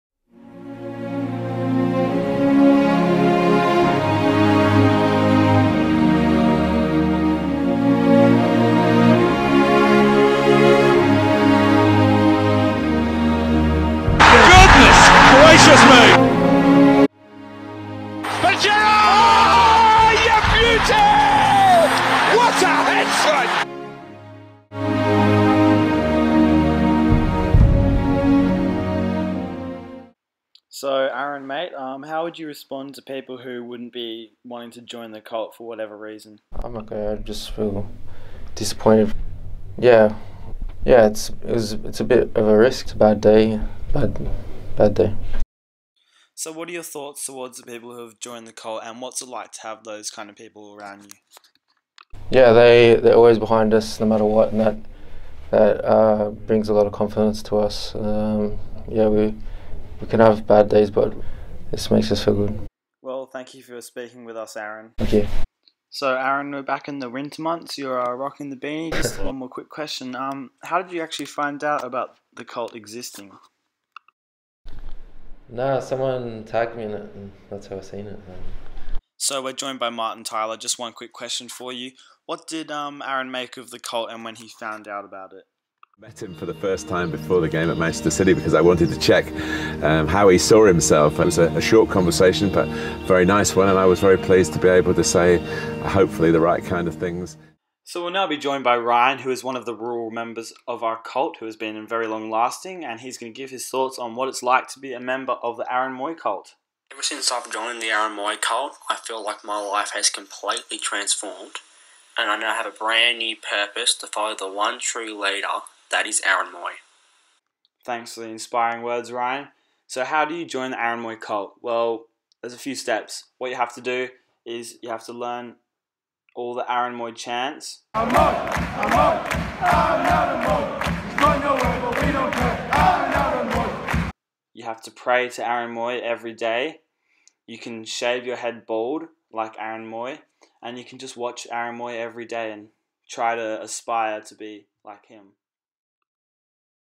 AARON MOOY DESTROYS INNOCENT PEOPLE
take a quick look at the destruction that takes place at the hands of aaron mooy.